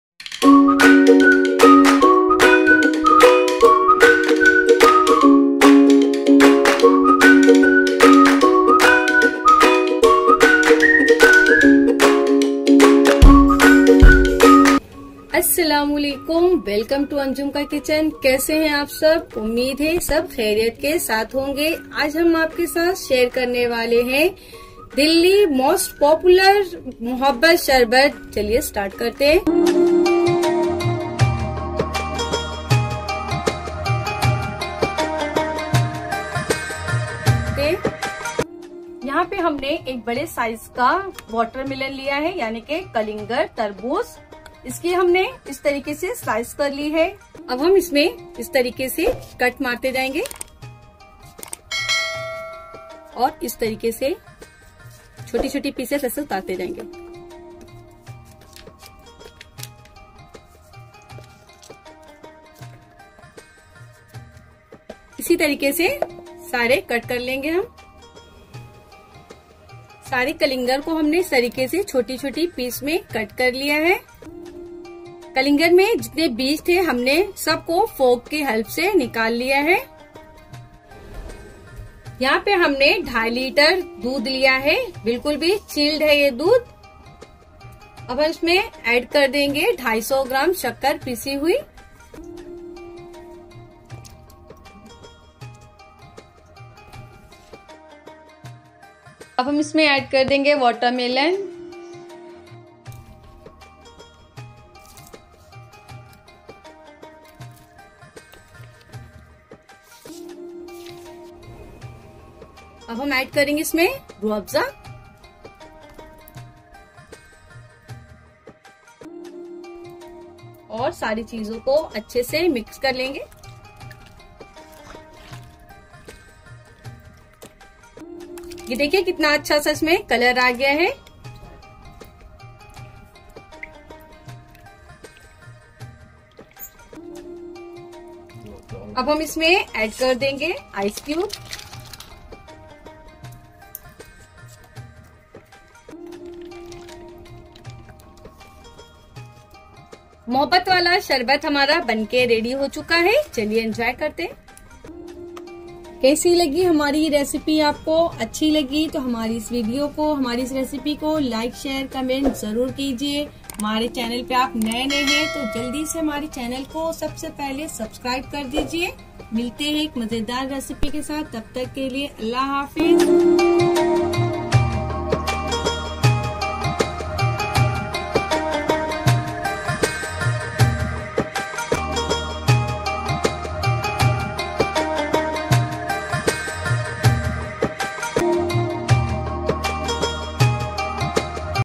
Sharbat-e-Mohabbat | Delhi Pyaar Mohabbat Ka Sharbat | Drink | Iftar Special | By Anjum ka kitchen
Aajj Main Apke Sath Sharbat-e-Mohabbat Ki Recipe Share Kari Hun Umeed Hai ApkopasandAyegi.

Hamaari Kuch Majedar Recipes Apko Dekhna Hai To Link Is Here.